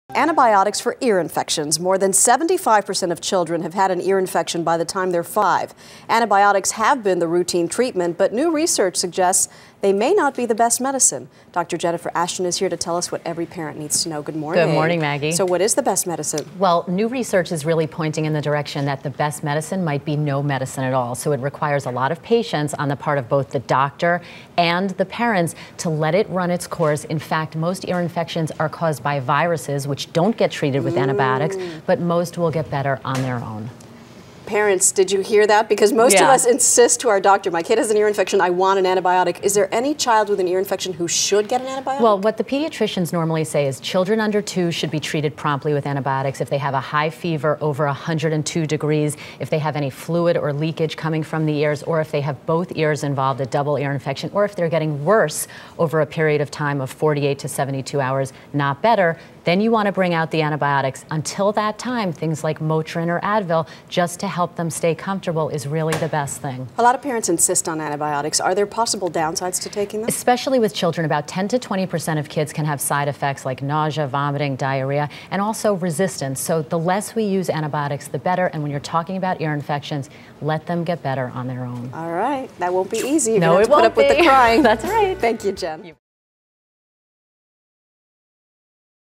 Ear Infections OK to Heal Alone
Ear infections in children are often caused by viruses and do not require antibiotics, a new study shows. Dr. Jennifer Ashton explains the findings.